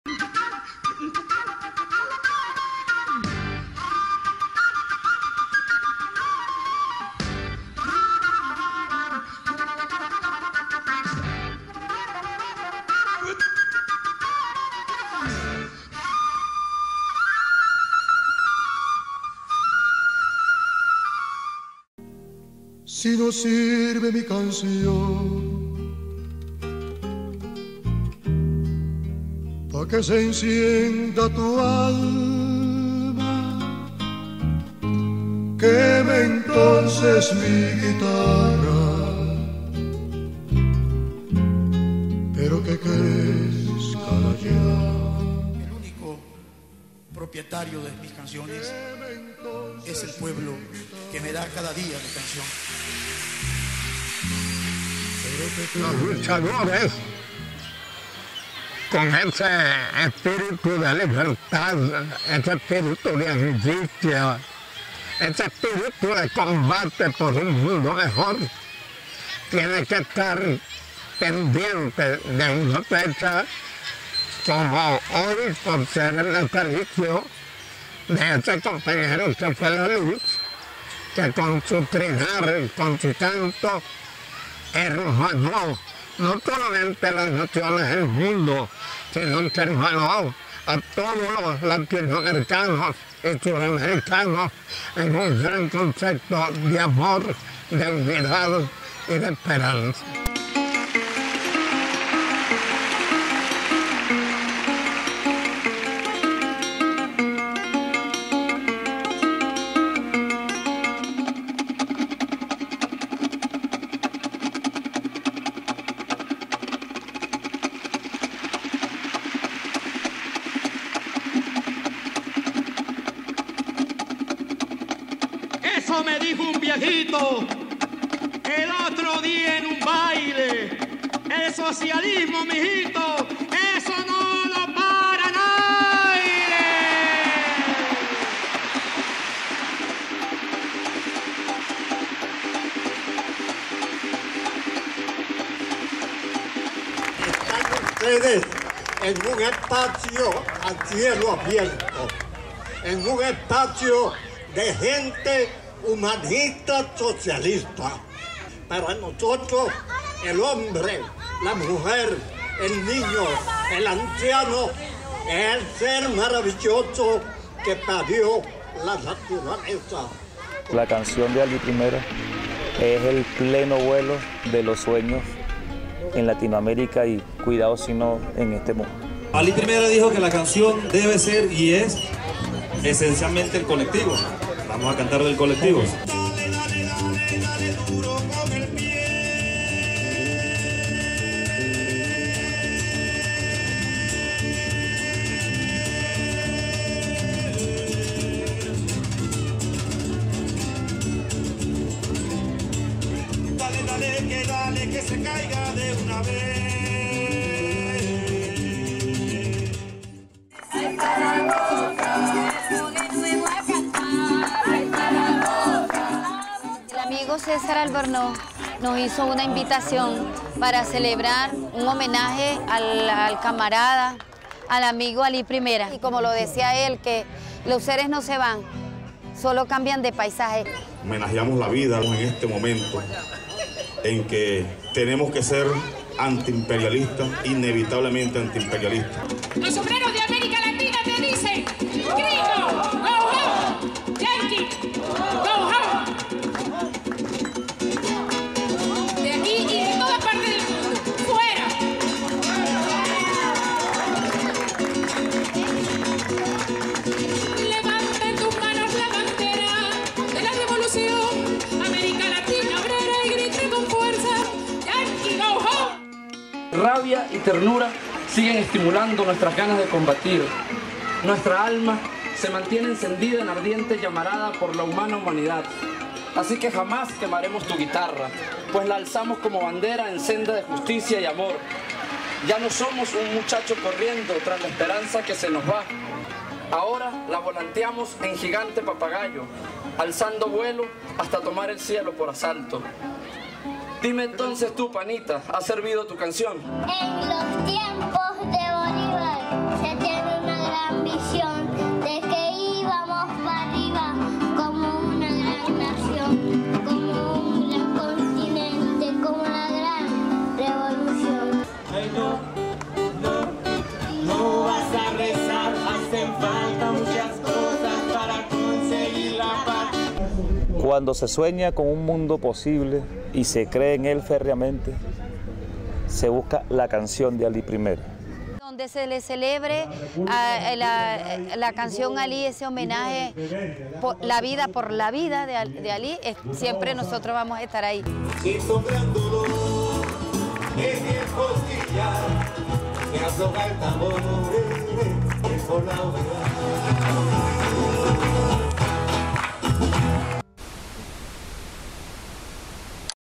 NotiCalles Cumpleaños Ali Primera El Rincon de los Muchachos
El viejo César, militante revolucionario, creador audiovisual y constructor del Parque de los Muchachos en la ciudad de Mérida, invitó al pueblo celebrar el pasado domingo 30 de octubre el Cumpleaños número 69 de su compañero Alí, en ese lugar mágico y espacial que logró reunir a diferentes colectivos culturales y movimientos sociales como: Tatuy Tv, Integración Universitaria, Abrapalabra, Cantores del ALBA, Surconciente, diferentes grupos musicales y culturales de la tierra de Sanare: Mujeres cantoras de Papelón, clavo y sabor, Dúo Las Alondras, El Morocho Catire y Carlos Falcón, Maestro Aurelio Colmenares y Leopoldo Pérez de la Cofradía de San Antonio de Padua, Bailadora Blanca Godoy, entre otros. Nos encontramos en este Homenaje para decirle a Alí y también a quienes nos adversan que, su guitarra no será quemada porque sigue encendiendo con su canto los corazones para continuar luchando.